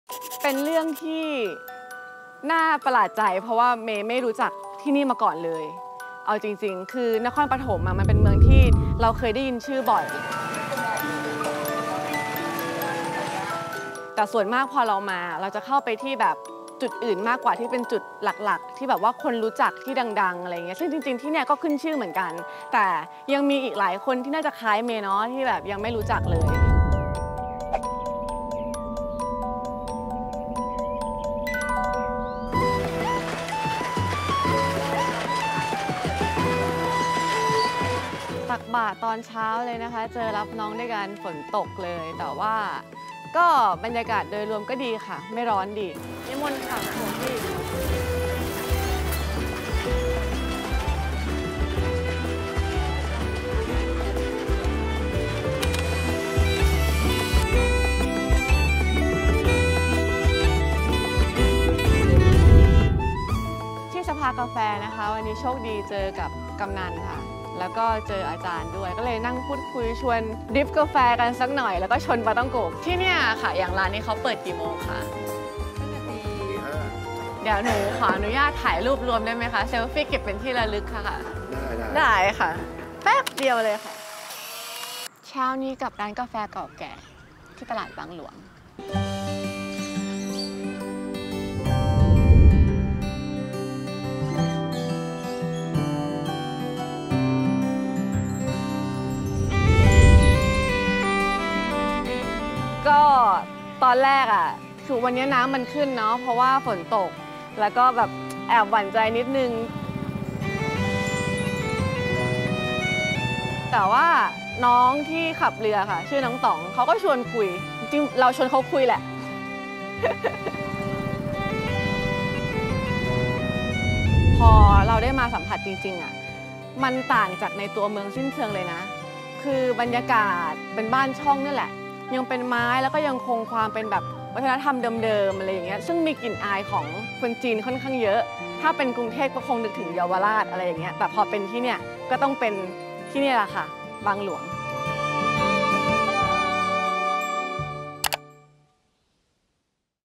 EP03 เพิ่งรู้ว่ามี! เสน่ห์วัฒนธรรมจีนที่ตลาดบางหลวง รศ.122 นครปฐม
นครปฐมจังหวัดใกล้กรุงแค่เนี้ย แต่กลับมีเสน่ห์ล้นเหลือ วันนี้เราจะพาไปทำความรู้จักชุมชนน่ารัก ตลาดบางหลวงร.ศ.122 ที่เที่ยวที่เต็มไปด้วยเสน่ห์ของวัฒนธรรมของชาติพันธุ์ไทย-จีน ลองไปนั่งสภากาแฟจิบกาแฟพูดคุยเรื่องราวจากคนในชุมชน ไปล่องเรือชมวิถีชีวิตชุมชน และอีกมากมายที่นครปฐมเล่าได้ไม่รู้จบ ตามไปชมกันเองเลยดีกว่า

คลิปวิดิโอนี้ถ่ายทำก่อนการระบาดของ COVID19 รอบใหม่

MAK-MiT Production Co.,Ltd